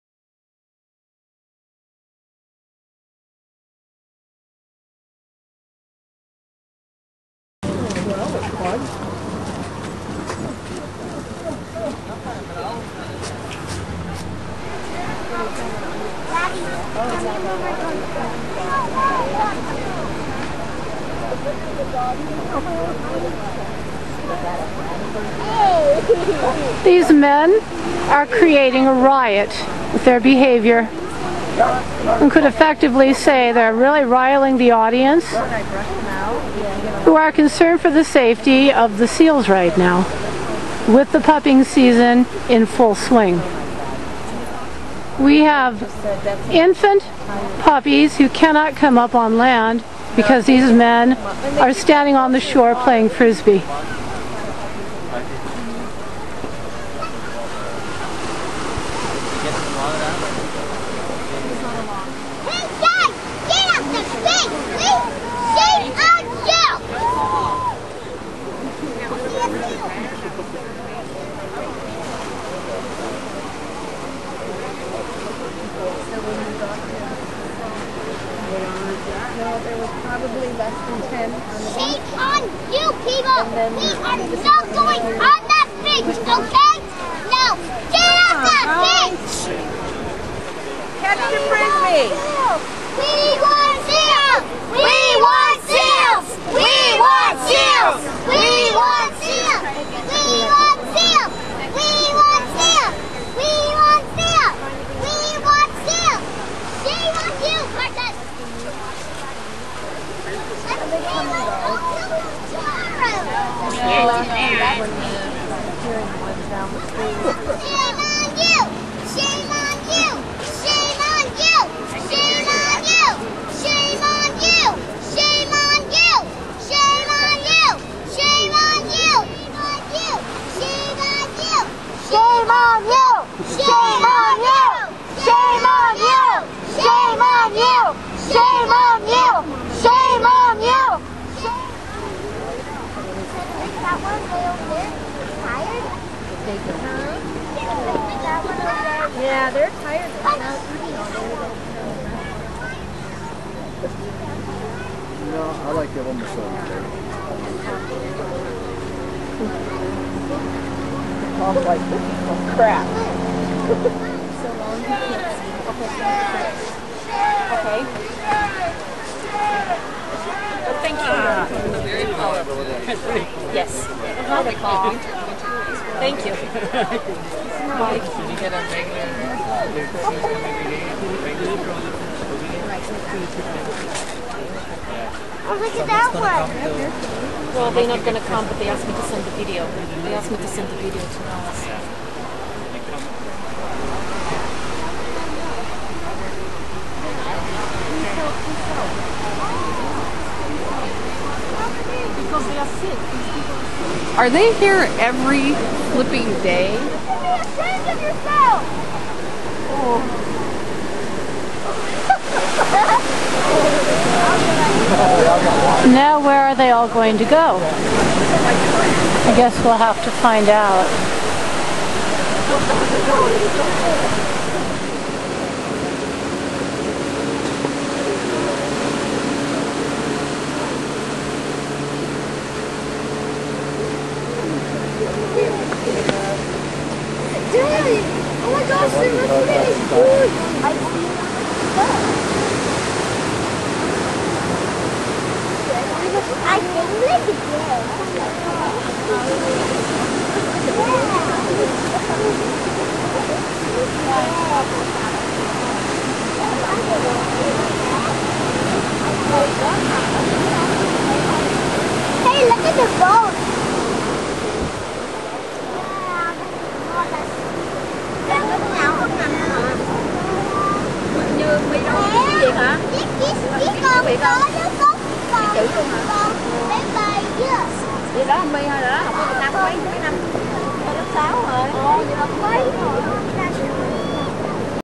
Pro access politics vs a 5 year old boy, Andrea Hahn
The Pro Access beach users came early this morning, flushing the older puppies and a pregnant female slowly away jut after 10;00 am;  but it was the intense interest they had with the sport of frisbie, played all morning and much of the afternoon except for when they were barbecuing, that had many highly upset, causing the two Rangers to call the lifeguards, who then called the police, who finally put an end to the frisbie marathon because it was inciting a riot, mostly on behalf of one little 5 year old boy, who simply wanted to see the Seals.